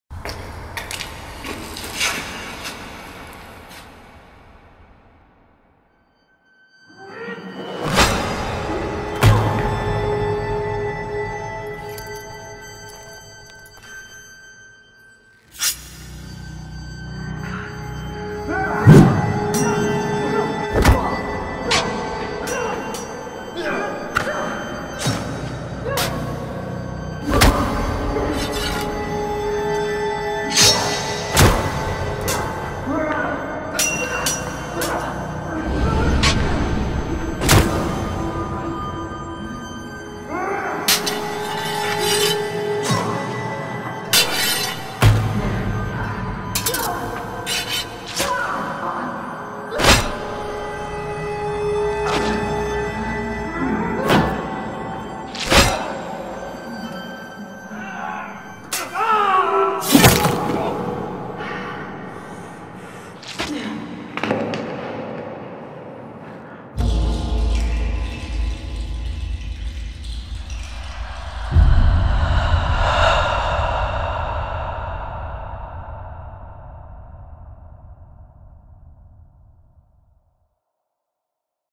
Concept Fight Scene
Fight design for a Viking concept action film.